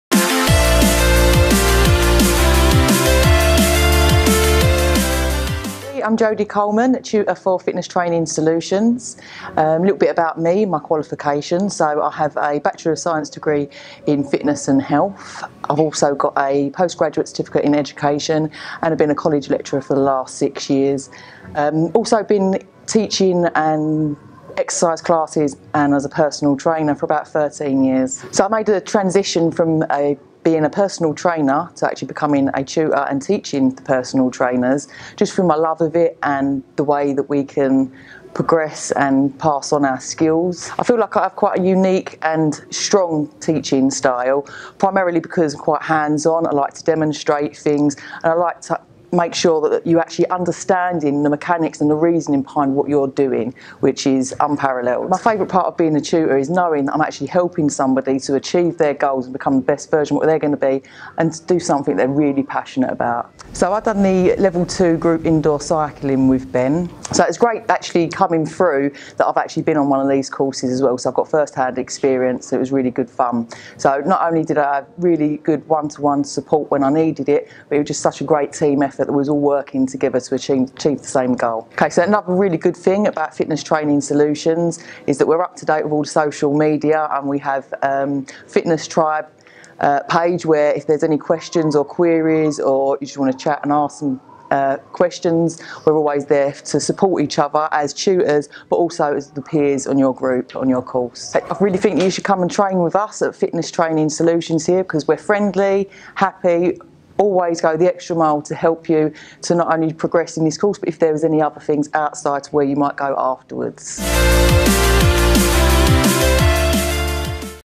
fitness training solutions tutor interview
Fts tutor Jodie Coleman explains why FTS are great at delivering fitness qualifications